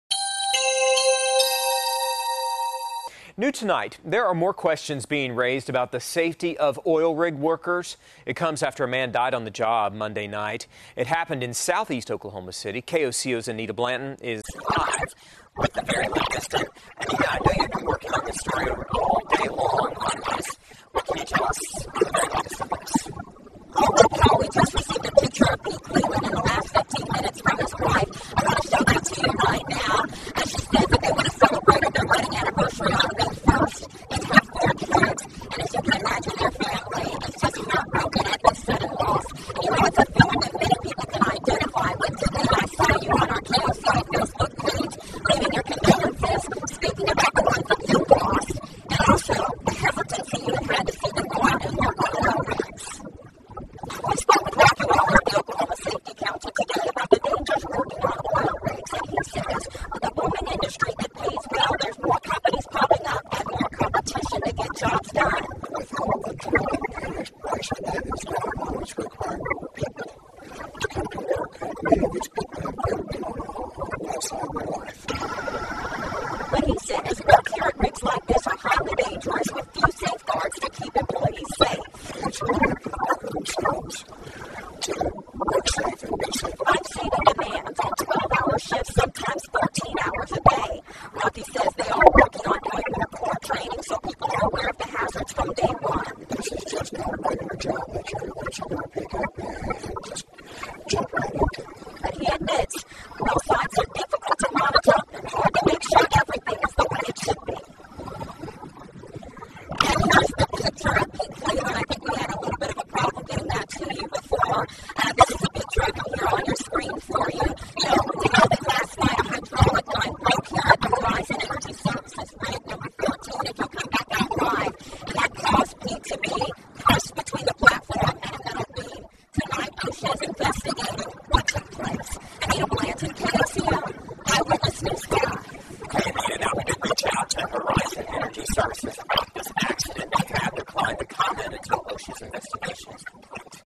Man Killed In Oil Rig Accident Identified
Authorities say Pete Cleeland was killed in Monday night's oil rig accident in SE Oklahoma City. KOCO Eyewitness News 5's Anita Blanton has the story.